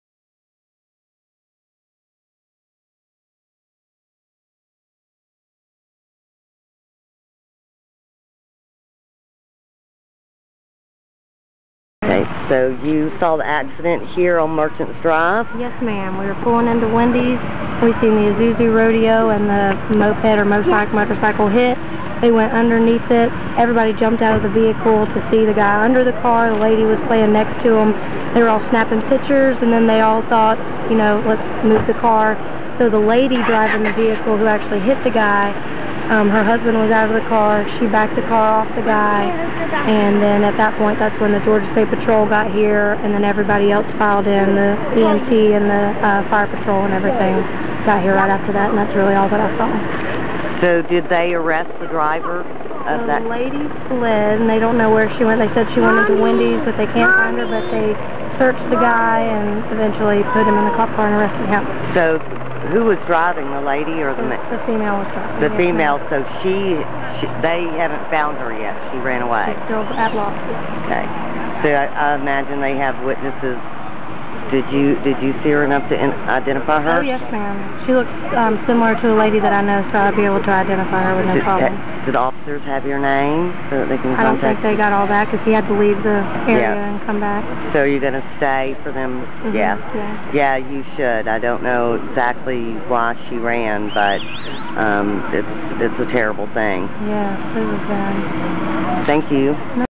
Motorcycle wreck out front of Wendy's in Dallas
LPPT gets an eyewitness report to a serious accident that left two motorcyclists injured and apparently authorities in search of the driver of the vehicle that caused the wreck.